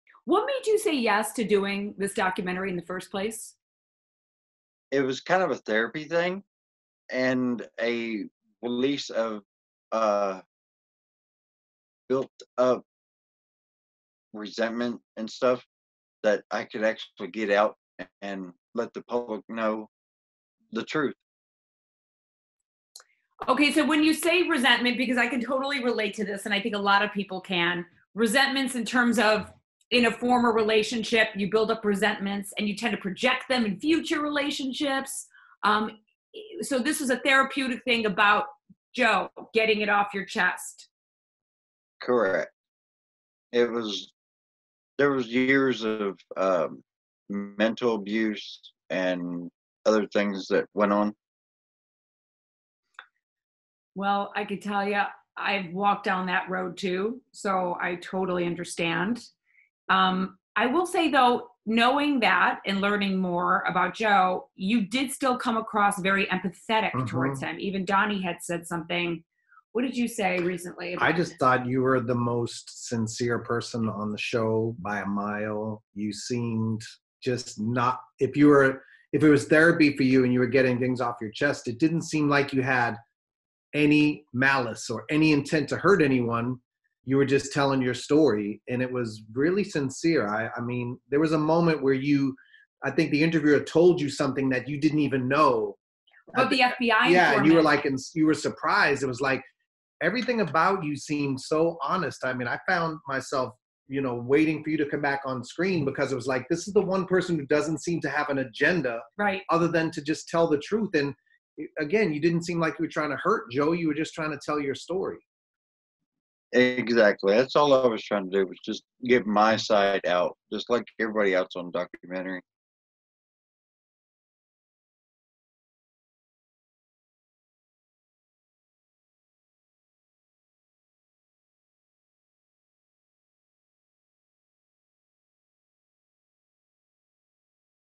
John Finlay Saw Filming 'Tiger King' as Therapy
Joe Exotic's first husband John Finlay tells Jenny McCarthy and Donnie Wahlberg why he participated in the hit Netflix docuseries 'Tiger King.'

Hear more from SiriusXM Stars on our app!